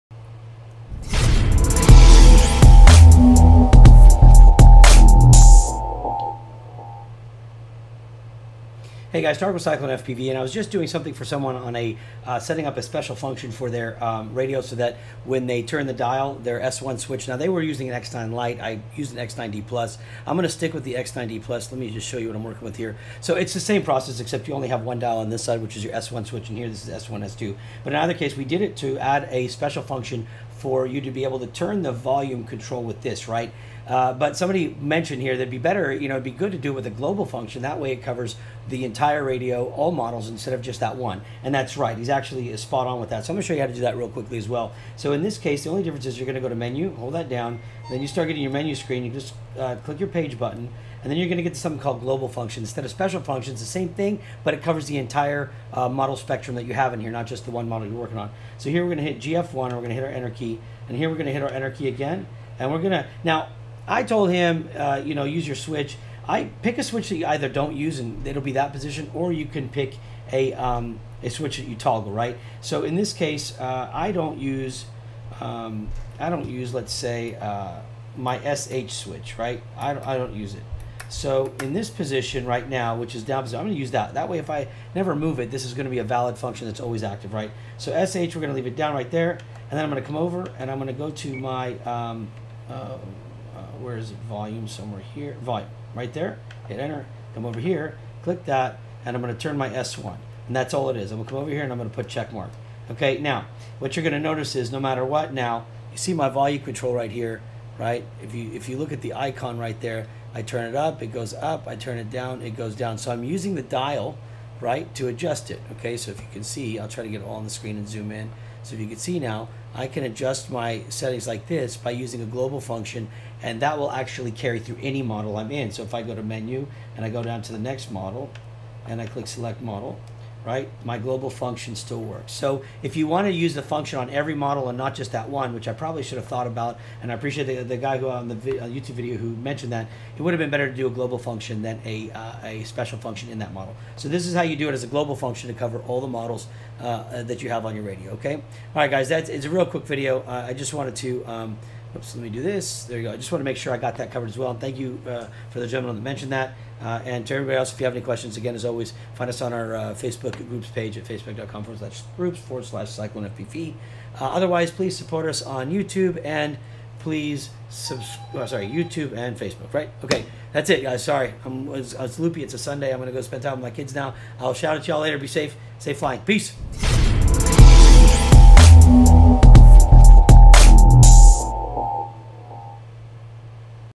How to set GF FrSky X9D Plus S1 for volume adjustment from Cyclone FPV
This video covers the FrSky Global Functions X9D Plus but can work on any model Taranis that you have. (thanks to John McCarty for reminding me to also use Global Functions)

Good luck and God Bless. Word of mouth will help us more than you could imagine.

Sincerely,

Tarek Maalouf
“Work Mule”
Cyclone FPV